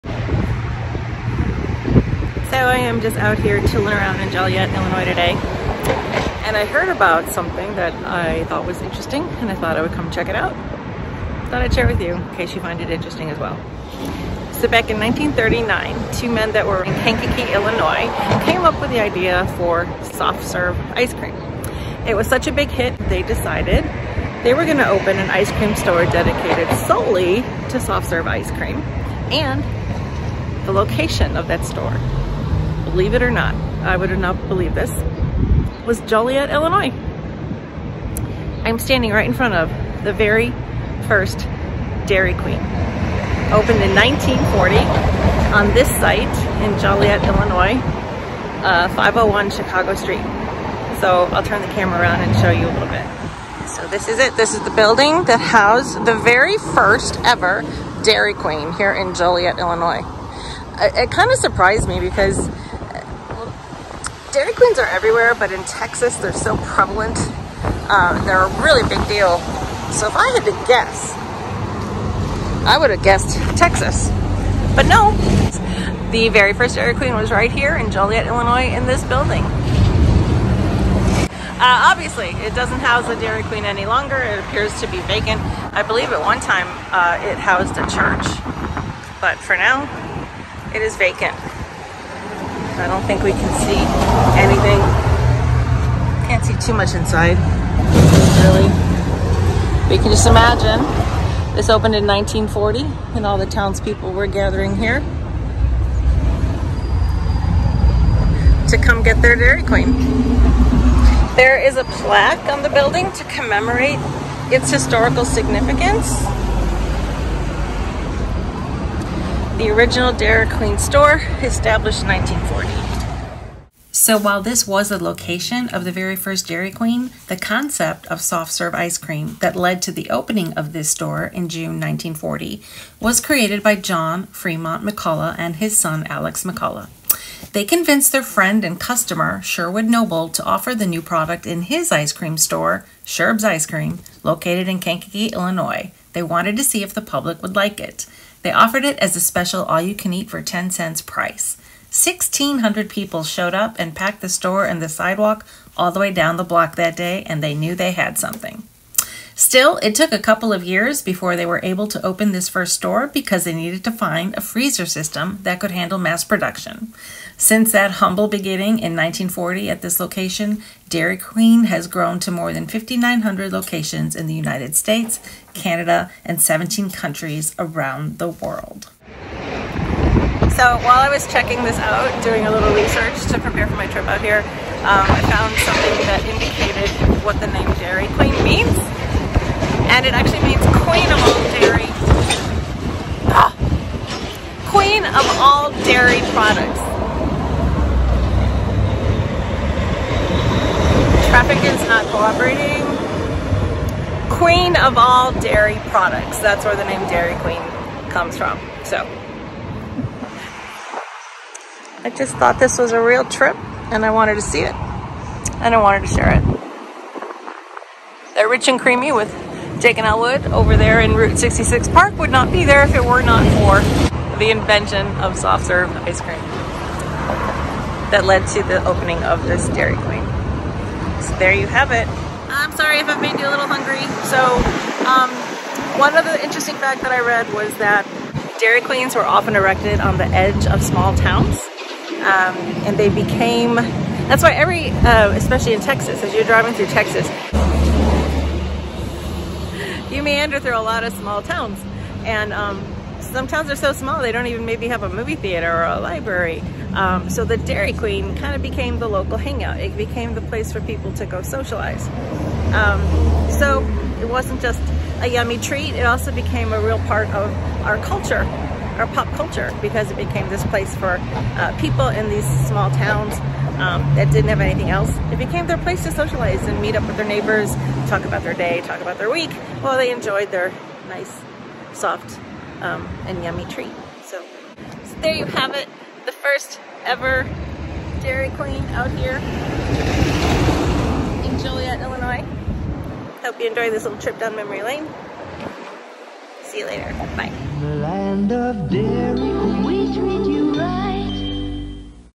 A visit to the worlds Very First Dairy Queen Ever!!!
Stopped by the location of the very first Dairy Queen ever!  Located in Joliet, Illinois, this Dairy Queen opened on June 22, 1940 and forever changed ice cream as we knew it!